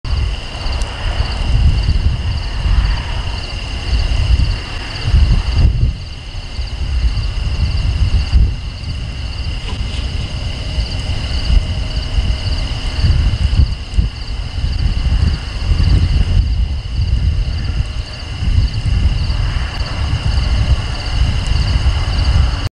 9/6/16 Someone departs the perch after 20 minutes - Boulder County Osprey Cam
Boulder County Osprey Cam